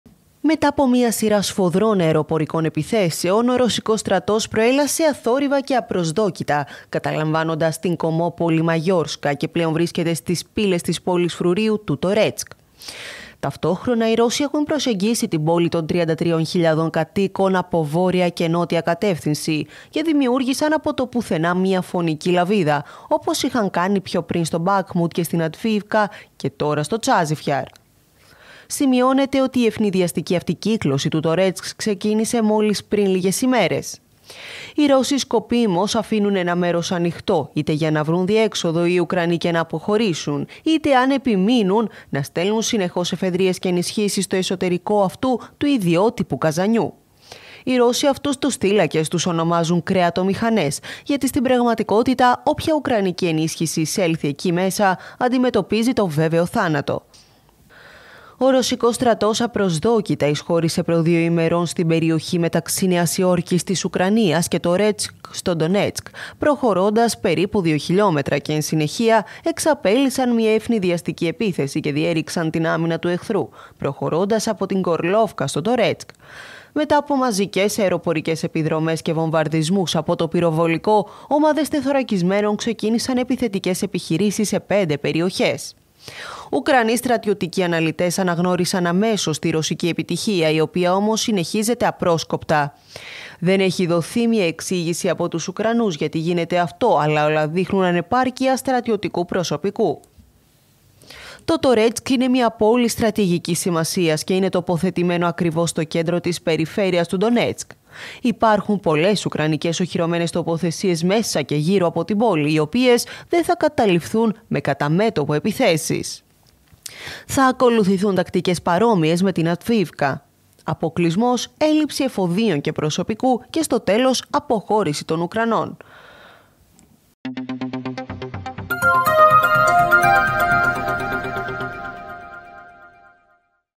Αυτή η πόλη είναι η πιο κρίσιμη για τους Ρώσους μετά το Μπακχμούτ και την Αβντιίβκα | DefenceNet TV
Μετά από μία σειρά σφοδρών αεροπορικών επιθέσεων με χρήση, ο ρωσικός στρατός προέλασε «αθόρυβα» και απροσδόκητα καταλαμβάνοντας την κωμόπολη Μαγιόρσκα  και πλέον βρίσκεται στις «πύλες» της πόλης-«φρουρίου» του Tορέτσκ.

Ταυτόχρονα οι Ρώσοι έχουν προσεγγίσει την πόλη των 33.000 κατοίκων από βόρεια και νότια κατεύθυνση και δημιούργησαν από το πουθενά μία φονική «λαβίδα» όπως είχαν κάνει πιο πριν στο Μπάκχμουτ και στην Αβντιίβκα και τώρα στο Τσάσιβ Γιαρ.

Σημειώνεται ότι η αιφνιδιαστική αυτή κύκλωση του Τορέτσκ ξεκίνησε μόλις πριν λίγες ημέρες.